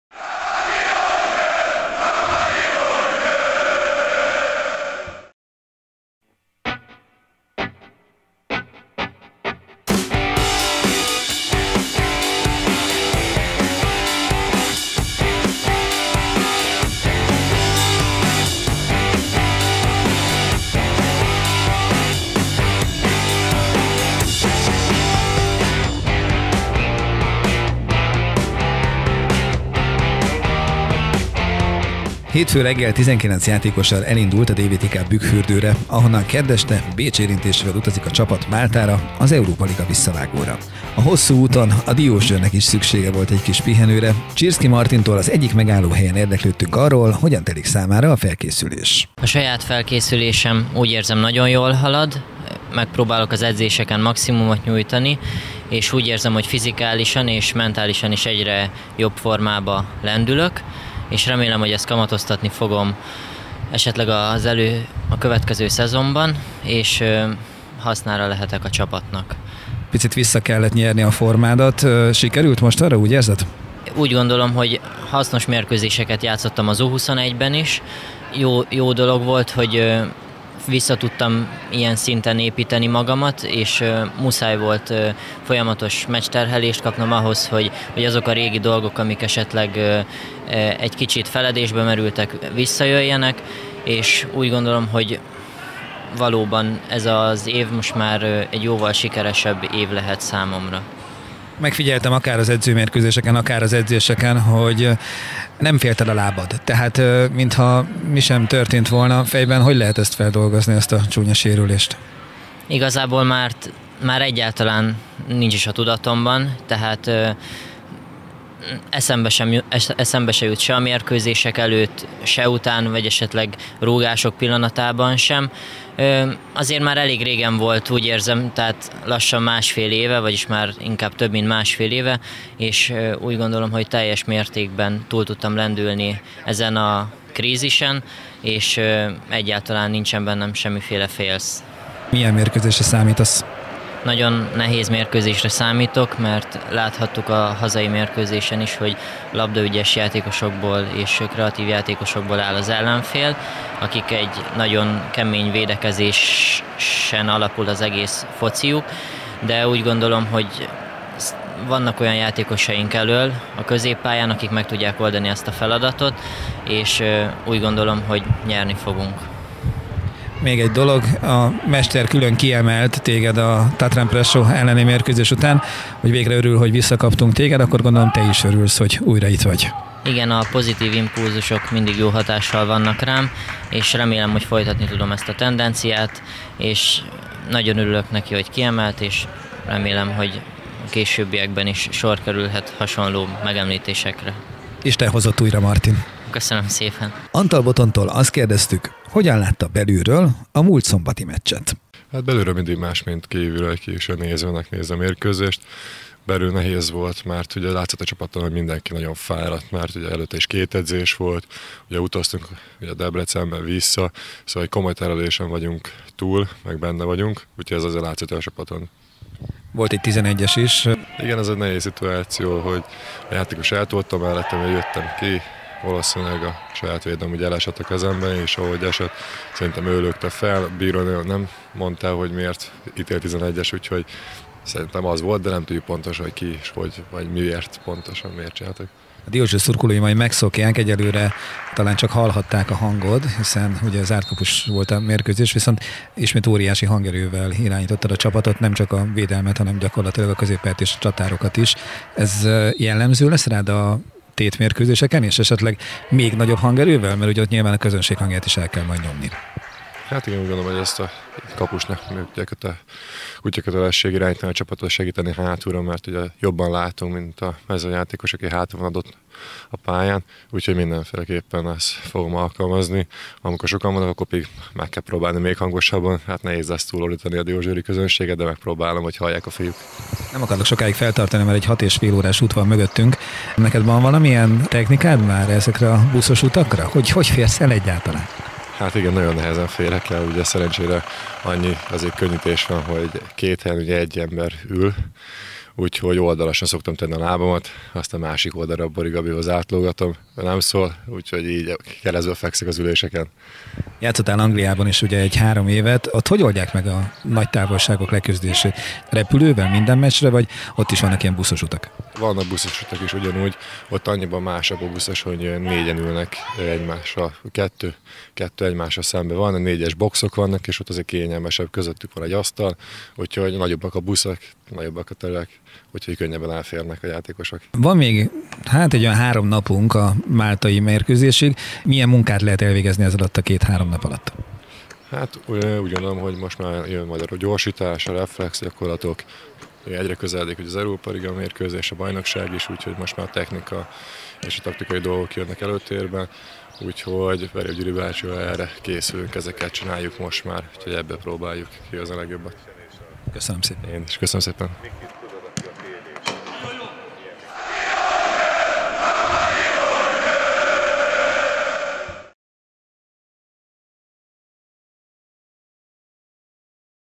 2014. július 7. Csirszki Martin és Antal Botond nyilatkozott a DVTK Tv-nek
Csirszki Martin sérülése már a múlté, a Tatran Prešov ellen már gólt is szerzett. Antal Botond pedig megosztotta, hogy egy hozzá hasonló magasságú sportoló hogyan él túl egy hatórás buszutat